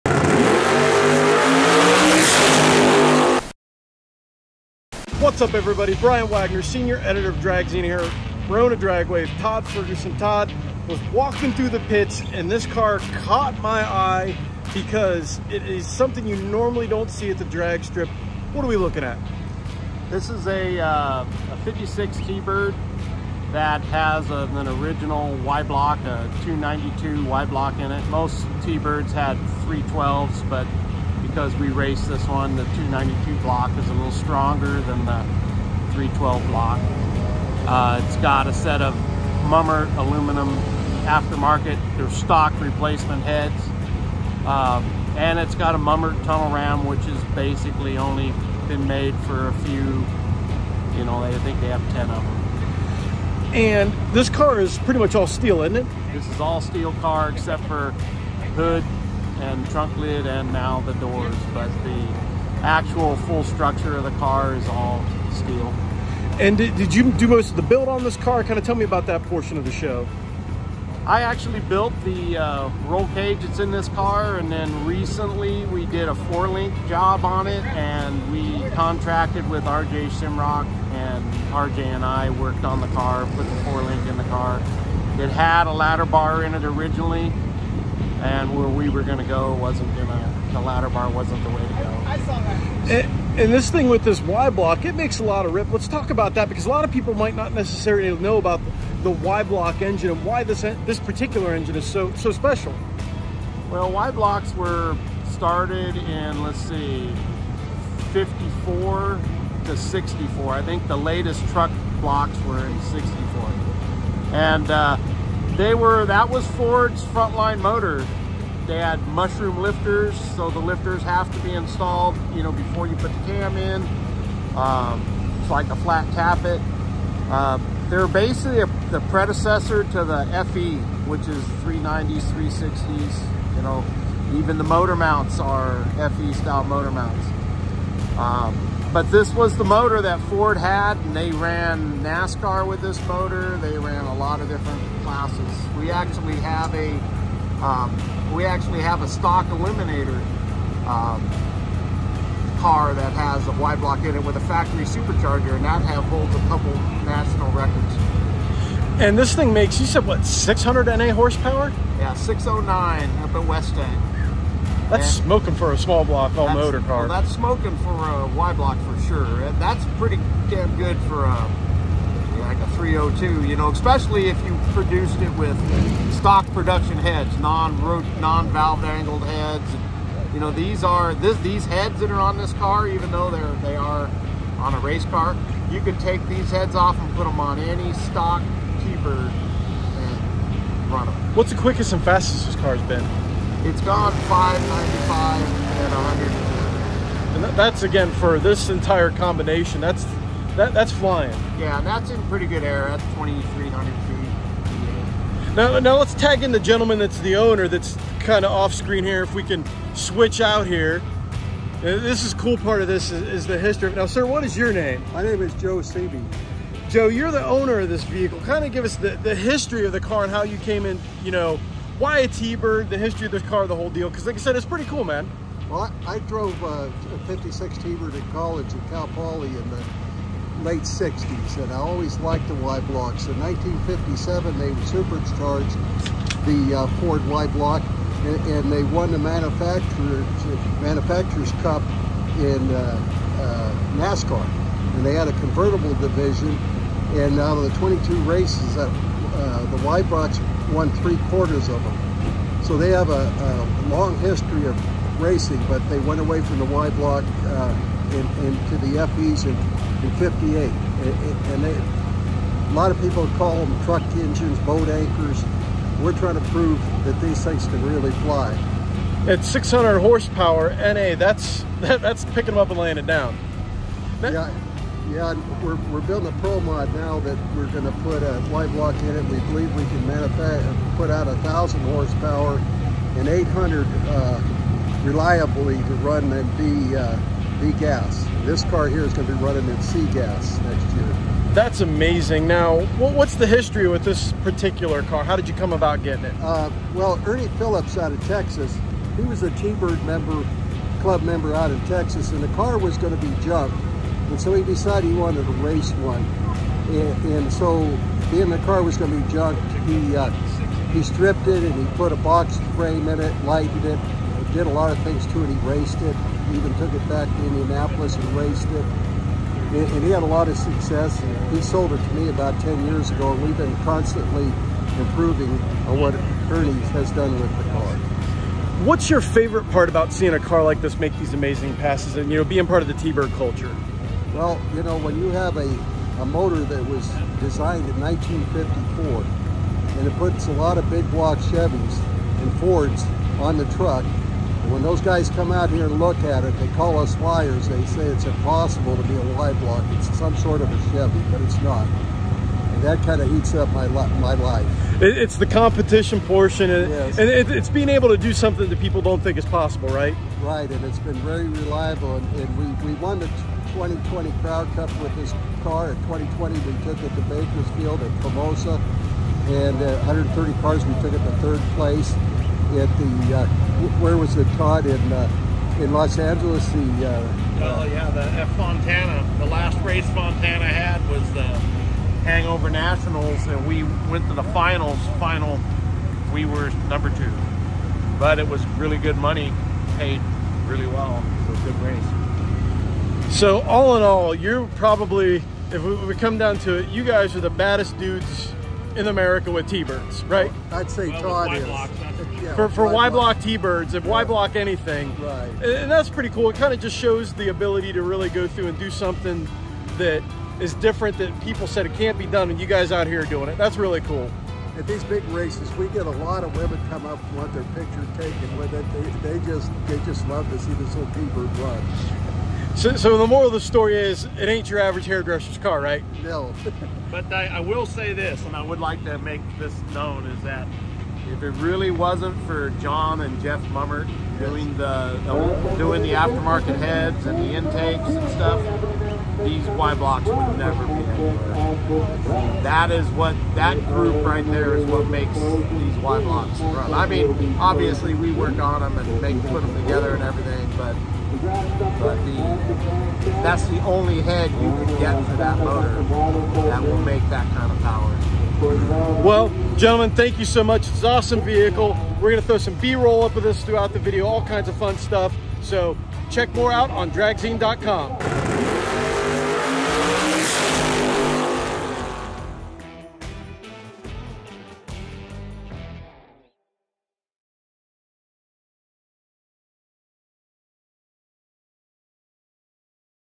The Bad Bird: Joe Cebe's Y-Block Powered T-Bird
Joe Cebe has always loved the Ford Thunderbird. This Thunderbird is powered by a nasty Ford Y-Block engine that makes over 600 naturally aspirated horsepower.

Power + Performance is the culmination of Power Automedia's suite of automotive magazines.